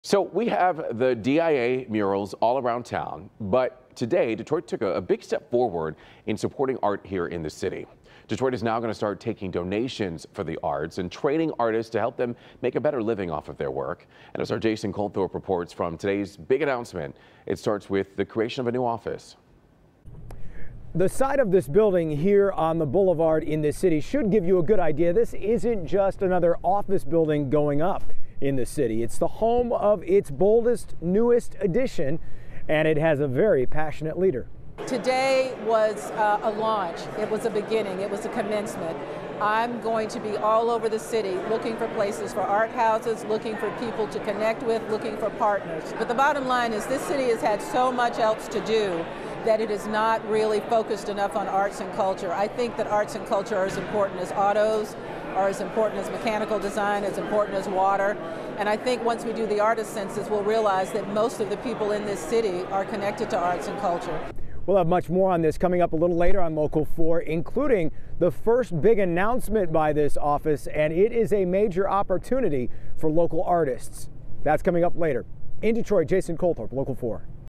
City of Detroit to introduce Arts, Culture & Entrepreneurship Office
Mayor Mike Duggan and the director of Arts and Culture, Rochelle Riley, unveiled a new comprehensive arts and culture plan that begins with the creation of the Office of Arts, Culture and Entrepreneurship (ACE) -- a hub for art and culture for the city.